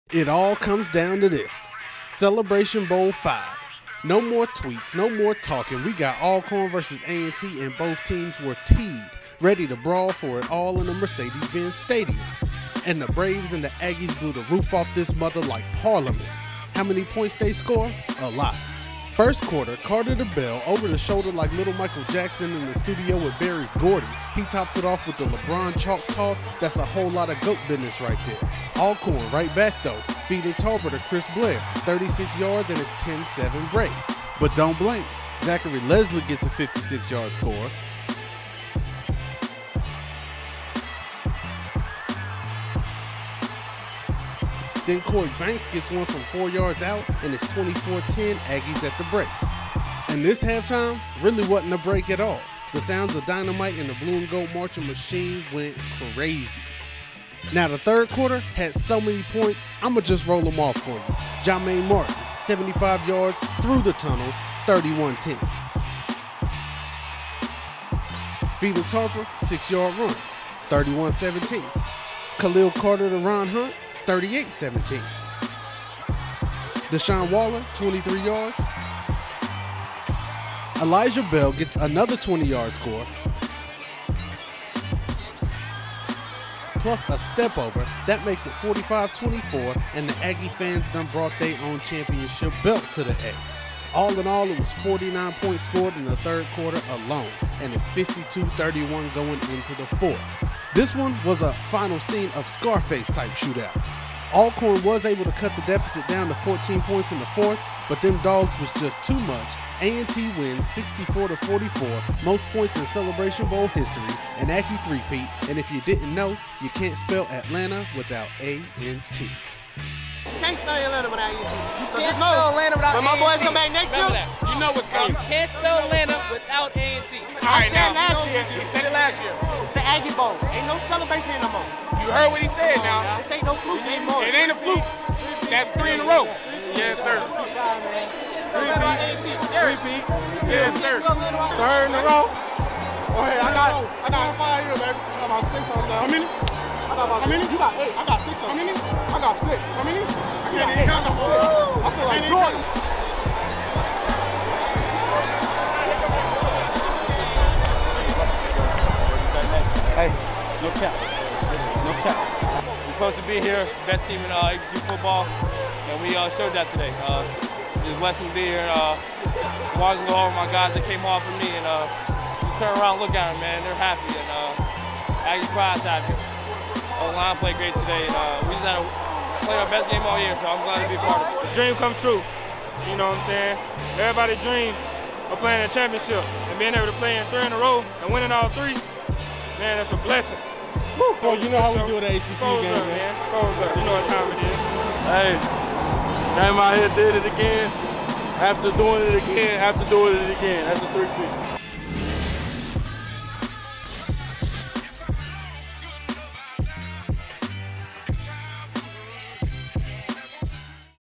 Celebration Bowl V: NC A&T vs Alcorn
For the third time in a row and the fourth time in five years, North Carolina A&T State University shines on the big stage with a win in the Celebration Bowl.